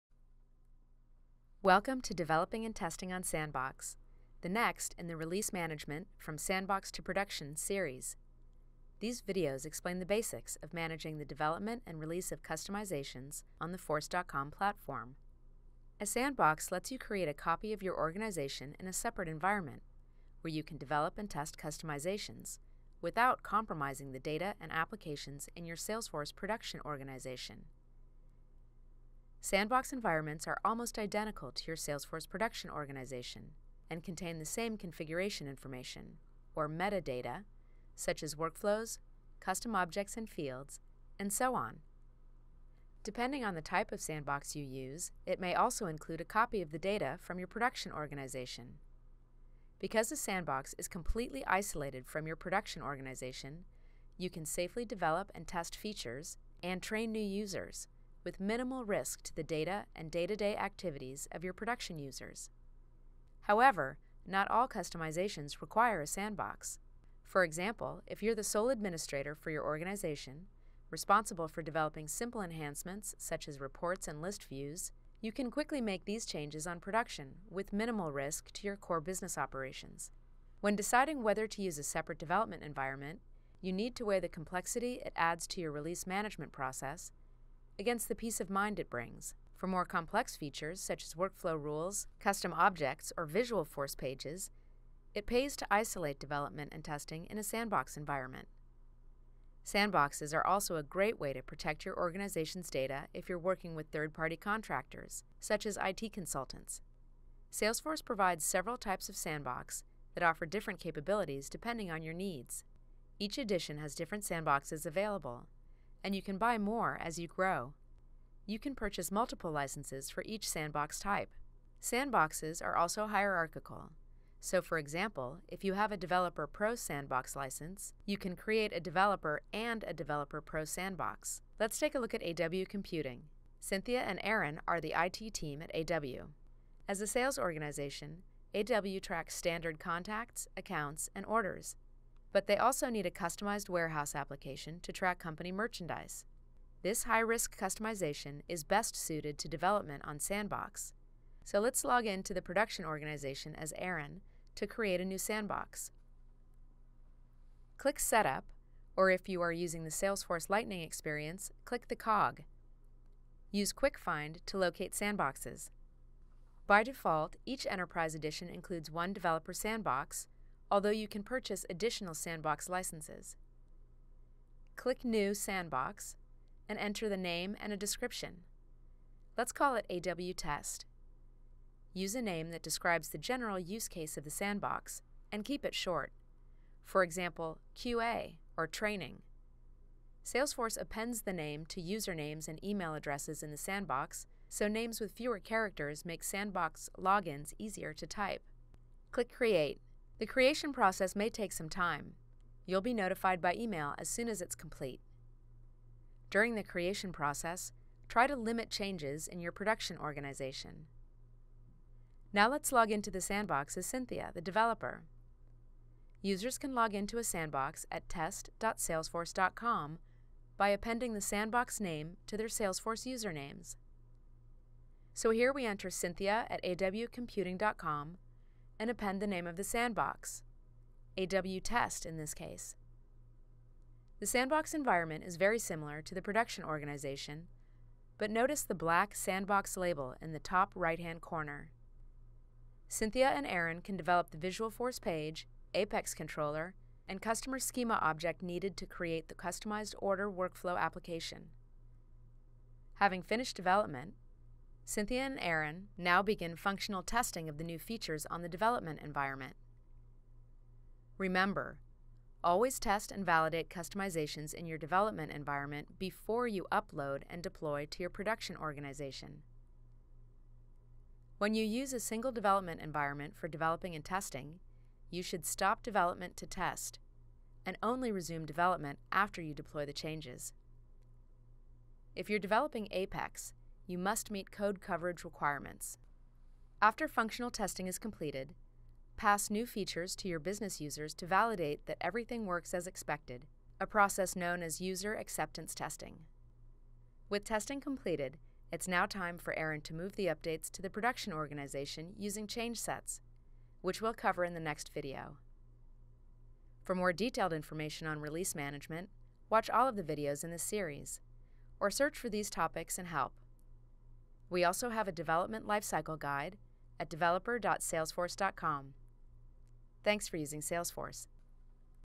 Release Management: Developing and Testing on Sandbox (Salesforce Classic)
This video describes the four types of sandboxes Salesforce provides, and explains how to create sandboxes to develop and test your applications. It’s the next in a video series that explains how to manage releases using Force.com’s point-and-click tools, a developer sandbox, and change sets, making it ideal for small-scale organizations with basic deployment needs. For more complex enterprise scenarios, such as building large-scale applications across multiple sandboxes, see the Development Lifecycle Guide.